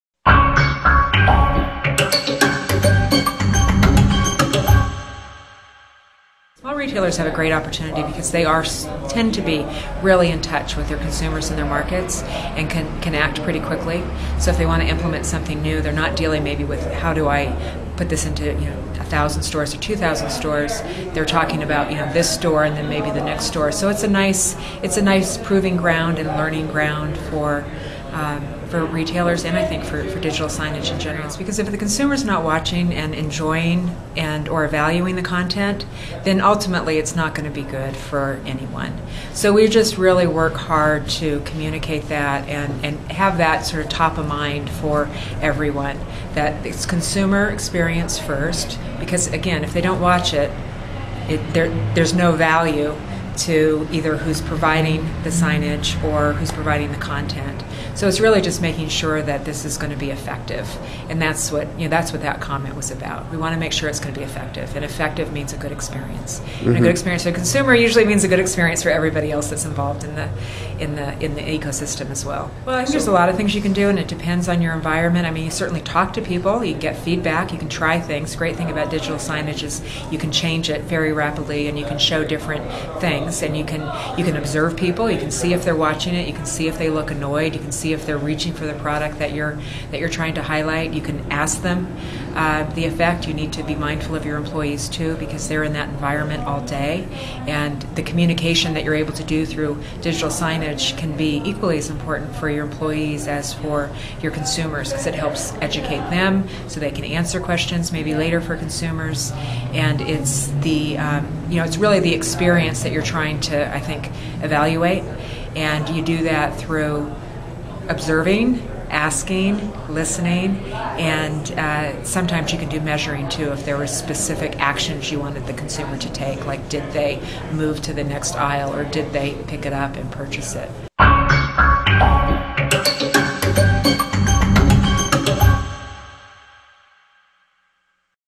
Digital Signage tips and tricks
Cathy Stauffer of Premeir Retail Networks gives pointers on how to effectively use digital signage.